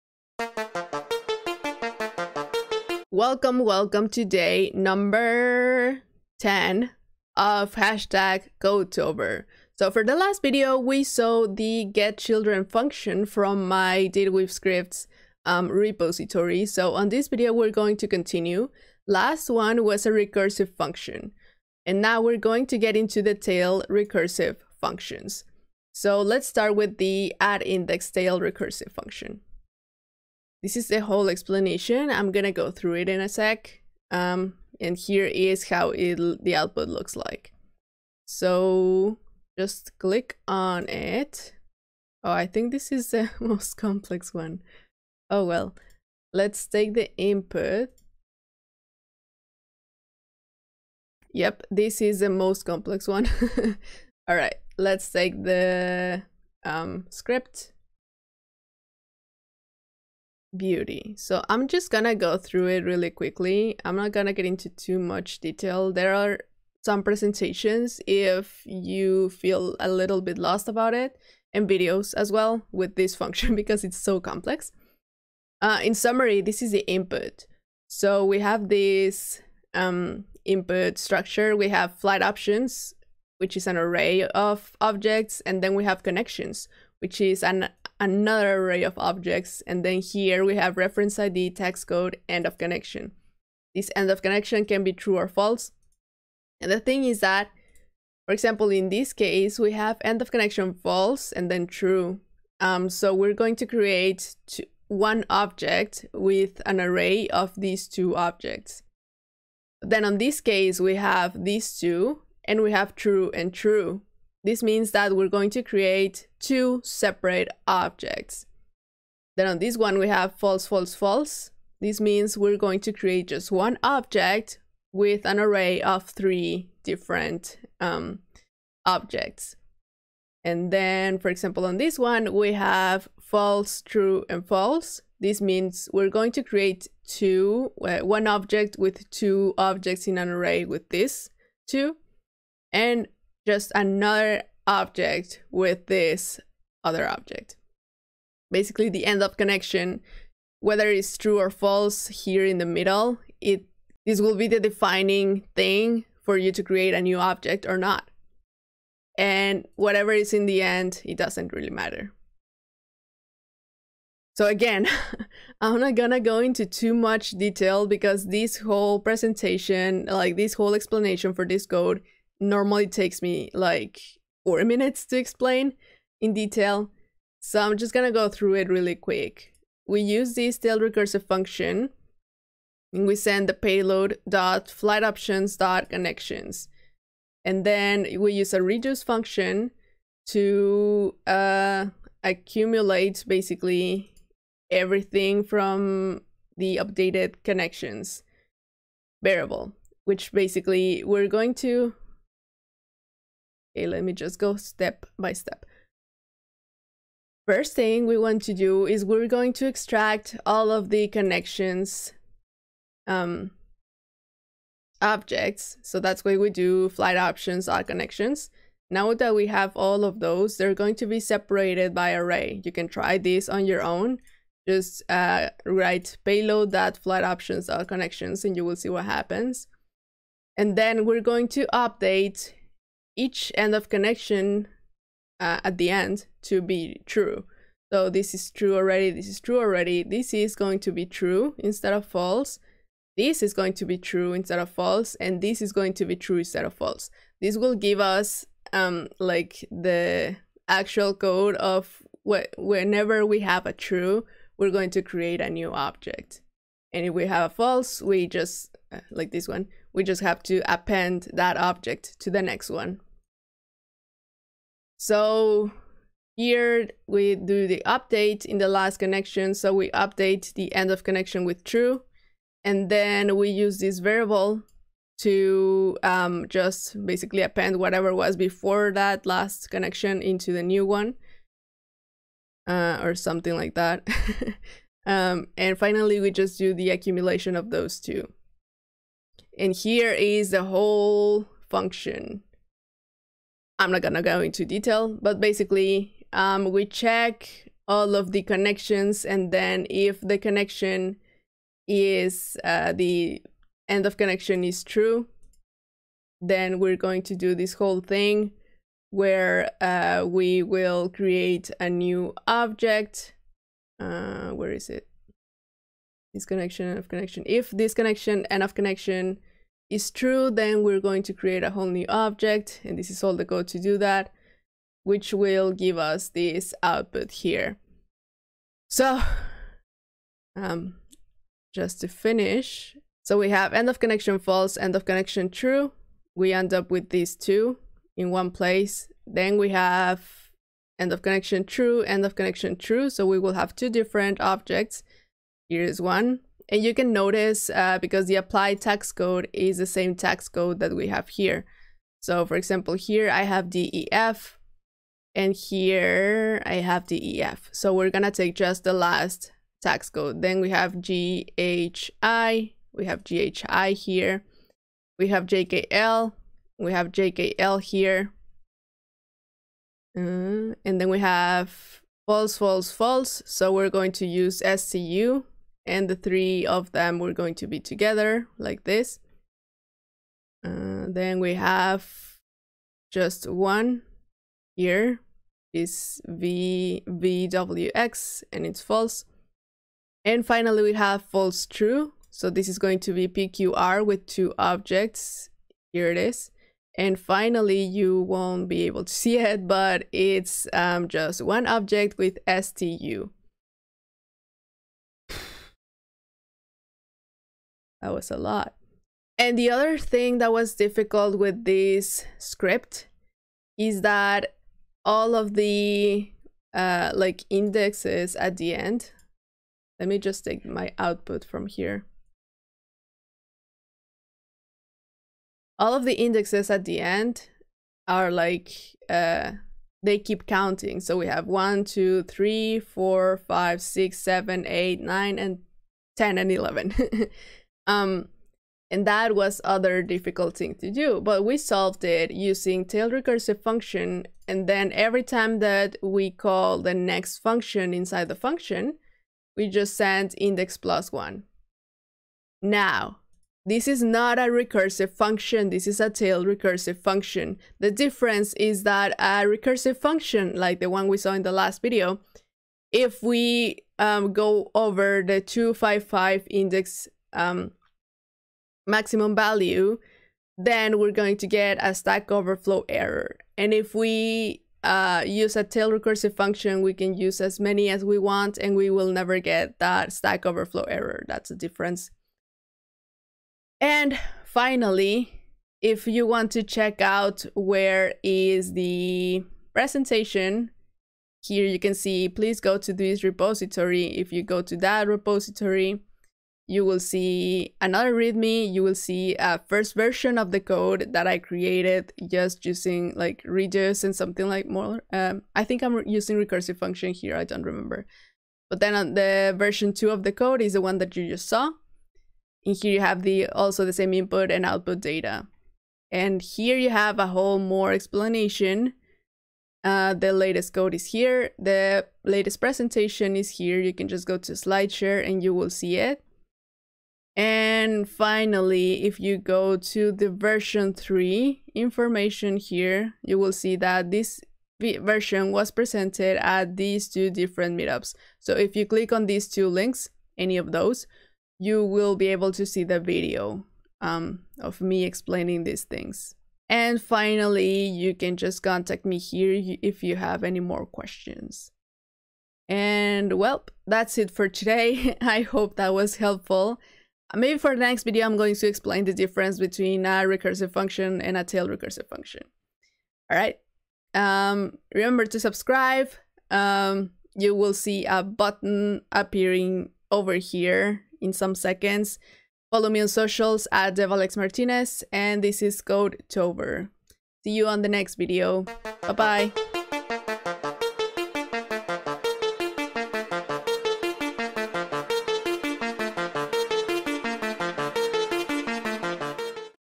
DataWeave Scripts Repo: addIndexTailRecursive tail recursive function | Codetober 2021 Day 10
Learn how to use my DataWeave Scripts repo in GitHub! In this video, we'll see the addIndexTailRecursive tail-recursive function.

Codetober 2021 Day 10
With Alexandra Martinez